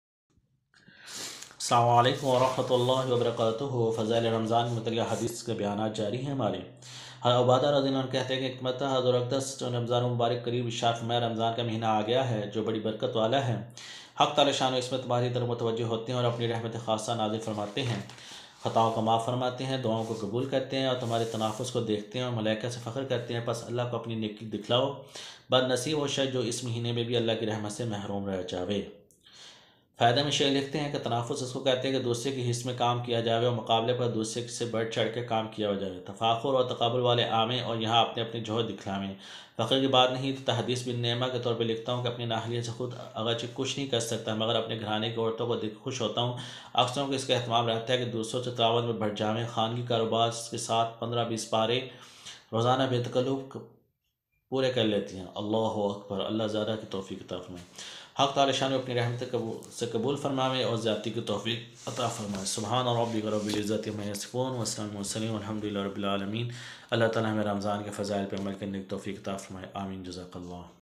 RAMADAN KA ISTIQBAAAL!RAMADAN KY FAZAEL!REHMATO KA MAHINA
رمضانُ المبارک کا مہینہ ا گیا ہے جو بڑی برکت والا ہے  حق سبحانہ وتعالیٰ اس مہینہ میں اپنی رحمتیں خاصہ نازل فرماتے ہے خطاؤں کو معاف کر تے ہےدعاوں کو قبول فرماتے ہے تمہارے تنافس کو دیکھتے ہے اور ملائکہ سے فخر کرتے ہے  پس تم اللہ کو اپنی نیکی دکھاؤں بد نصیب ہے وہ شخص جو اس مہینے بھی اللہ کی رحمت سے محروم ہے۔۔
اللہ کی رحمت متوجہ ھے اپنے بندے کی طرف۔
برکت والا مہینہ ہے
گناہوں کو معاف کرانے کا مہینہ ہے